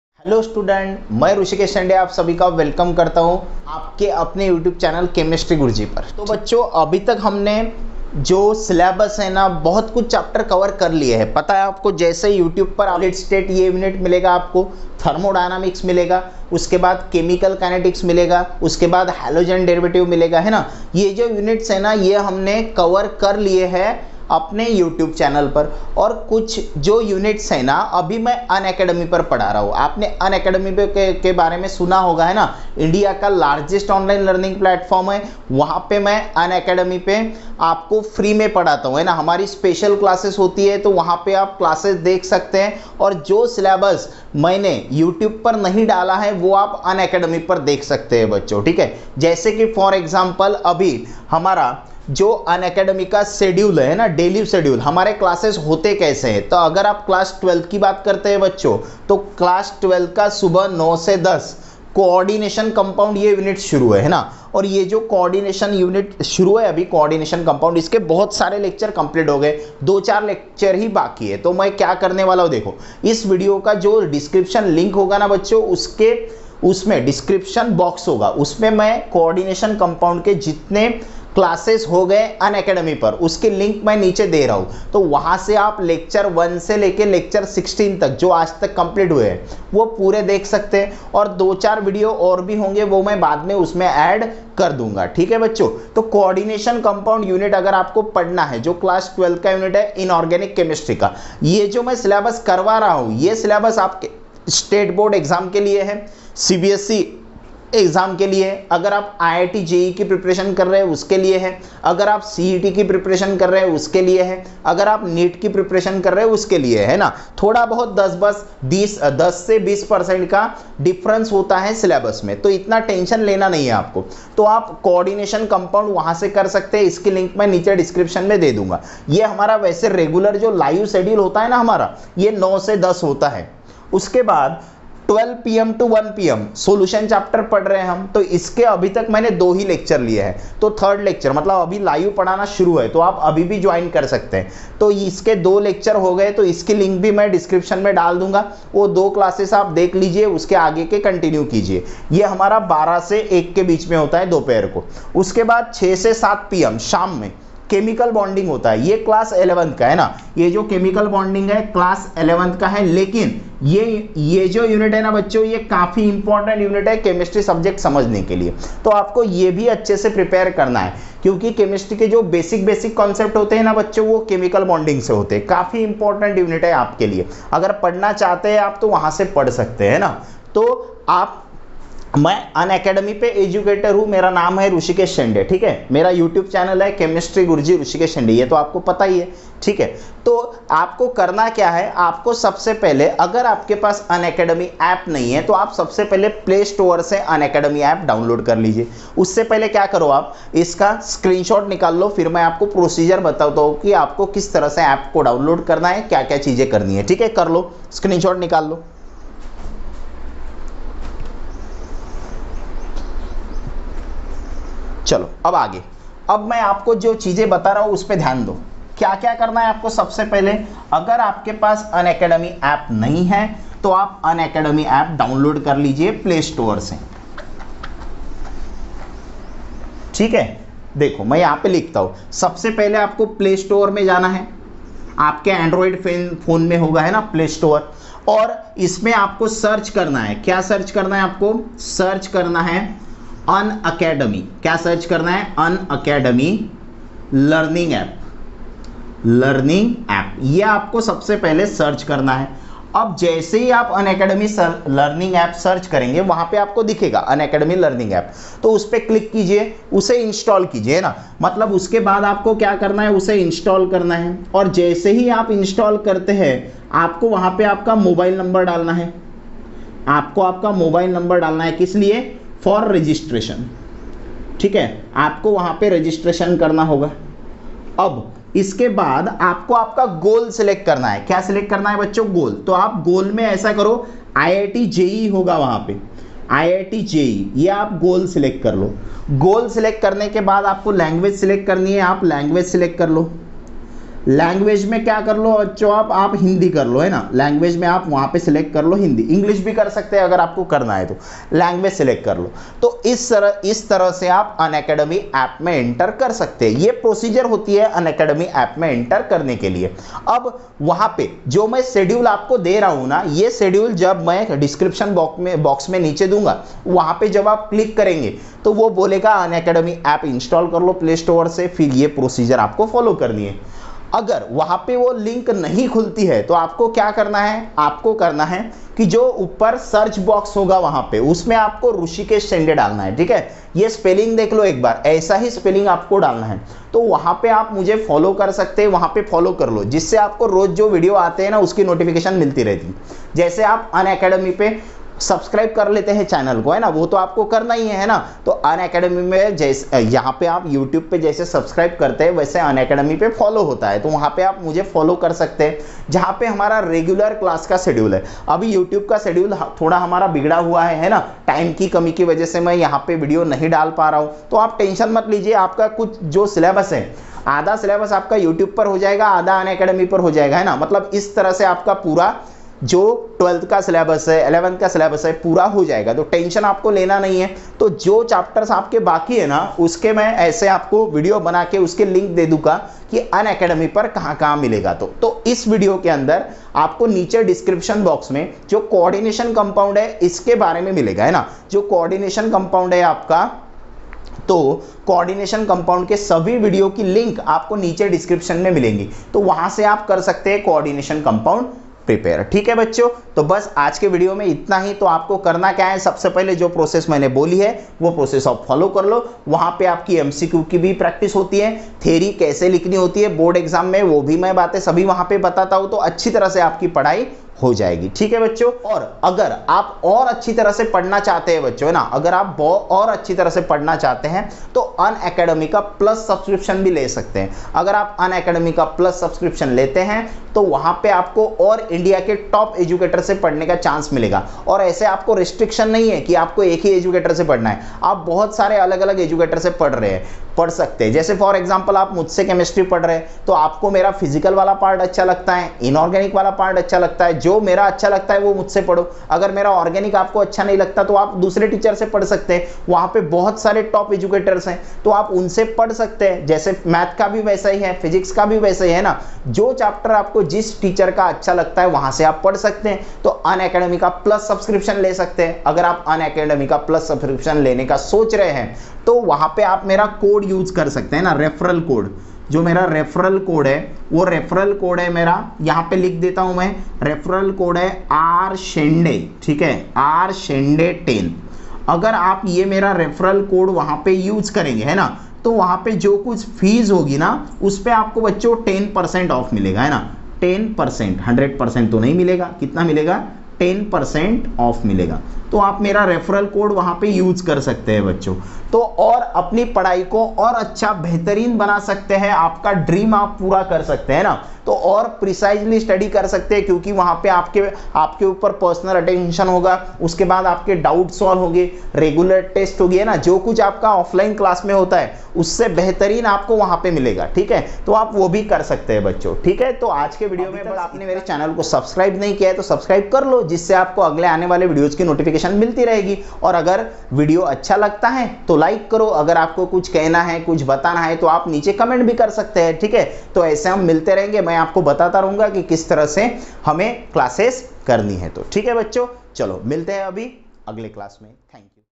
Coordination Compound | Class 12 | State Board|CET|JEE |NEET| Rhushikesh Shende | Unacademy Educator
Rhushikesh Shende (Unacademy Educator) teaches in IIT JEE completed Coordination Compound Chapter of Class 12.

The link of classes are given below (Lecture wise):
                                                                                                           Coordination Compound (Total Chapter – Class: 12)